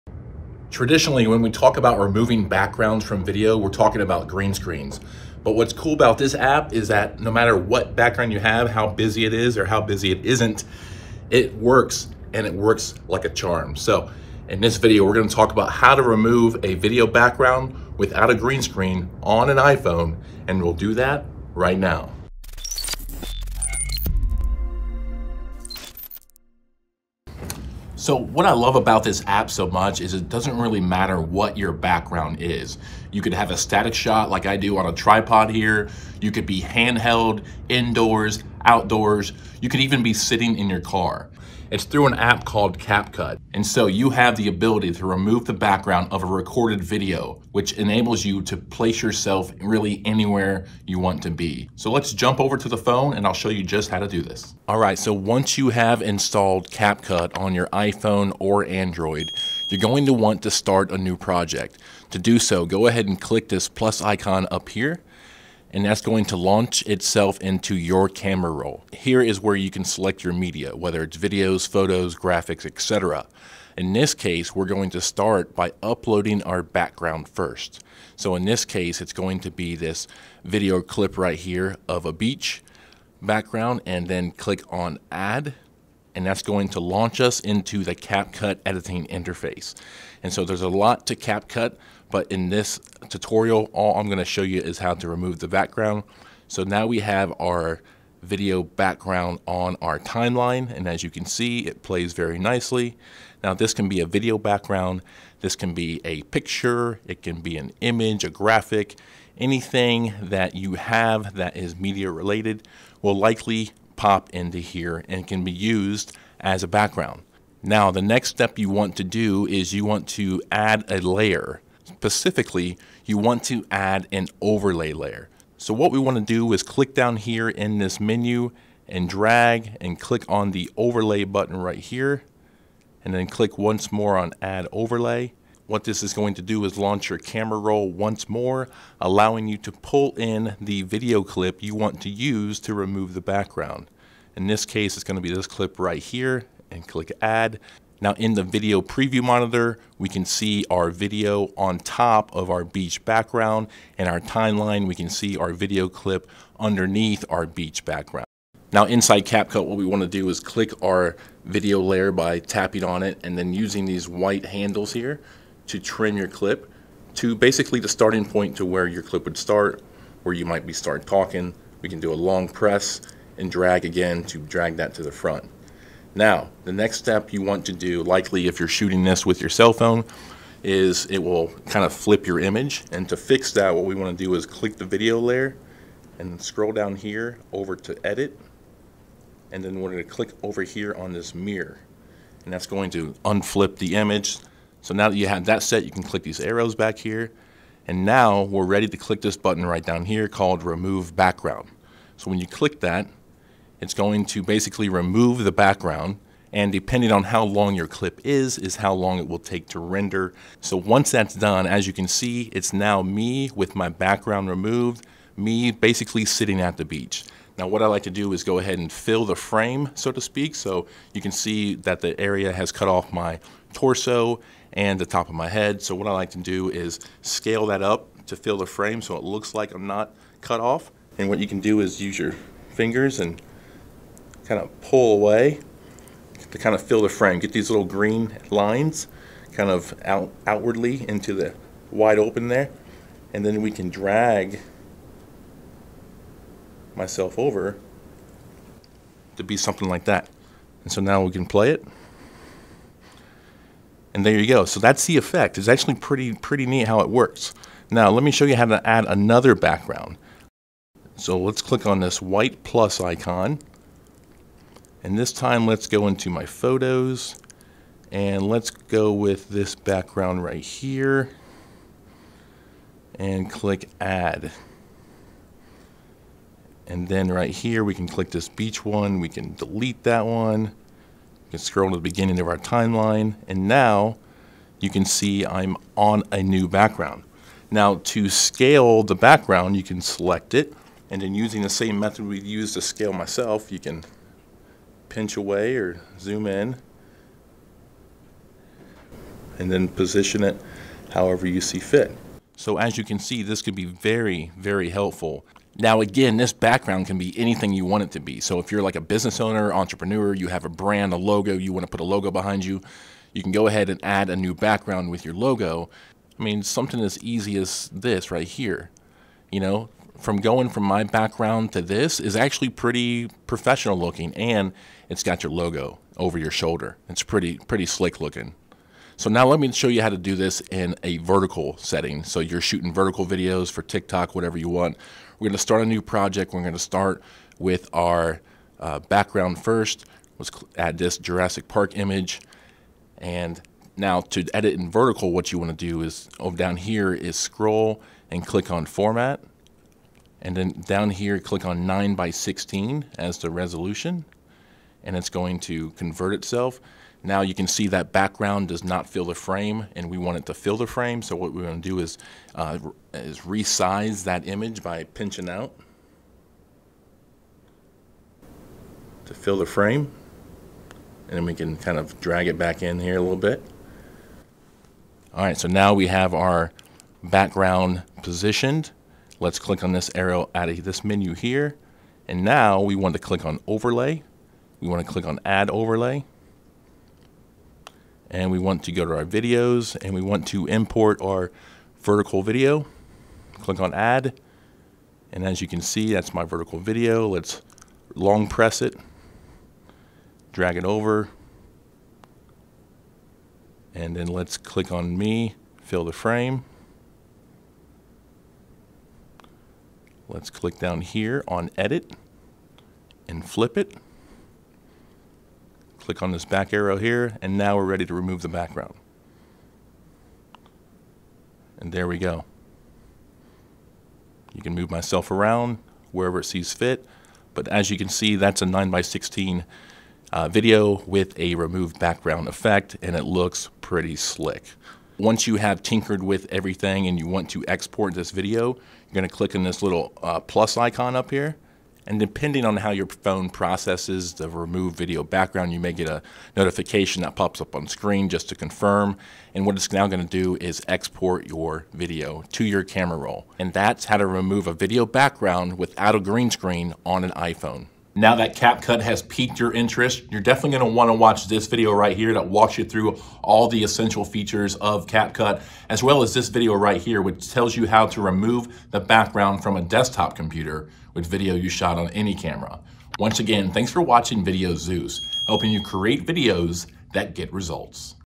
How To Remove Video Background Without Green Screen on iPhone
In this video, you'll learn how to remove a video background without a green screen on iPhone and Android. Removing or changing your background opens up a lot of new opportunities to create content without the worry about your background. The effect can be accomplished with a new app mentioned in the video. The app is compatible with iOS and Android devices.

Purchase Professional Zoom Backgrounds with Your Logo On It!